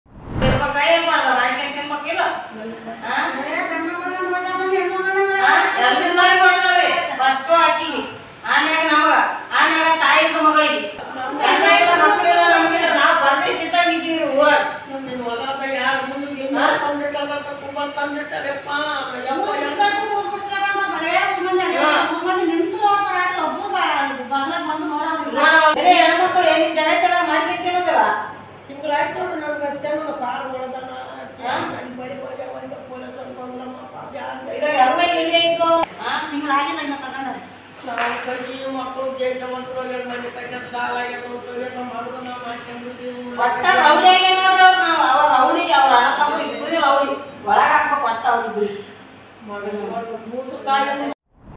fight between two groups over a trivial cause : ಕ್ಷುಲ್ಲಕ ಕಾರಣಕ್ಕೆ ಎರಡು ಗುಂಪುಗಳ ನಡುವೆ ಮಾರಾಮಾರಿ | HSR
Karnataka | ಕರ್ನಾಟಕ
Kannada | ಕನ್ನಡ